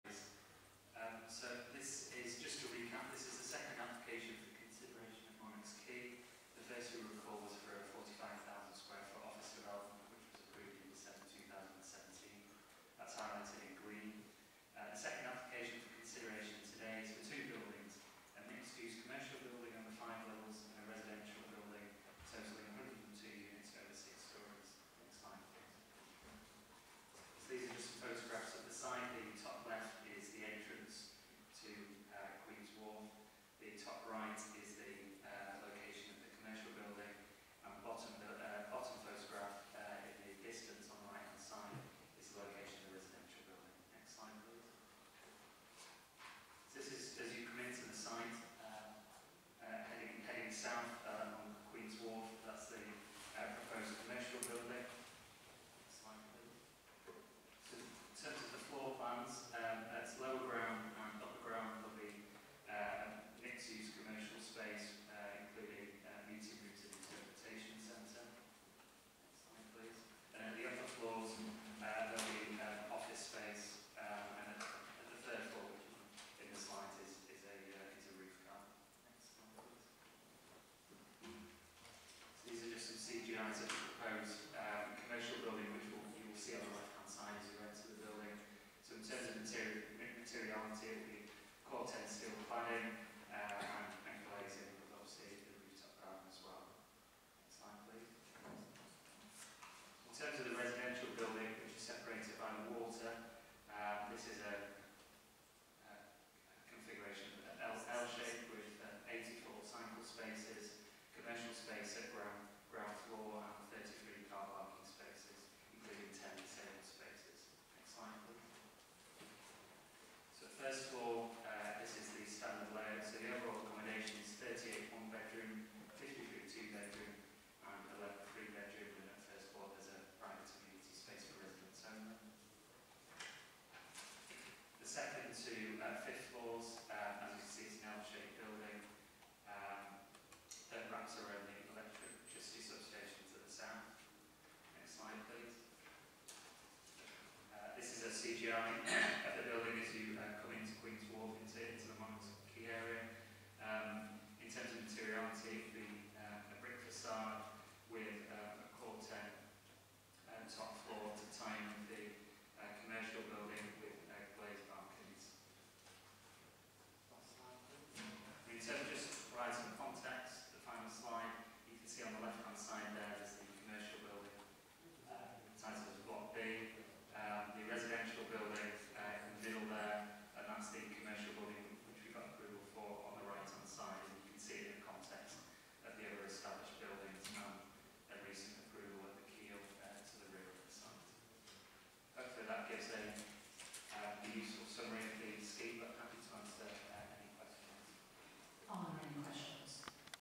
Planning Committee (Liverpool City Council) 24th April 2018 slides 1 of 8 17F/3261
Council Chamber, ground floor, Liverpool Town Hall, High Street, Liverpool, L2 3SW

These are the slides shown by the agent.

The agenda and reports for this meeting can be found on this

4. Land at Monarchs Quay, Liverpool L3 4FP (Riverside Ward)

To consider a recommendation that Application No. 17F/3261 in respect of Land at Monarchs Quay, Liverpool L3 4FP (Riverside Ward), to erect a mixed use development, including a Commercial Building, comprising an Interpretation Centre with flexible A1, A2, A3, A4, D1 uses (Lower and Upper Ground Floors) and B1 Office use (Upper Floors),  and a Six Storey Apartment Block, comprising of 102no. one, two and three bedroom apartments, with ground floor commercial use and 33no. car parking spaces and associated landscaping / public realm works, be approved subject to a Legal Agreement and the conditions proposed by the Assistant Director – Highways & Planning.

Additional documents:

Item No. 4 - Site Plan - Land at Monarchs Quay, Liverpool L3 4FP (Riverside Ward)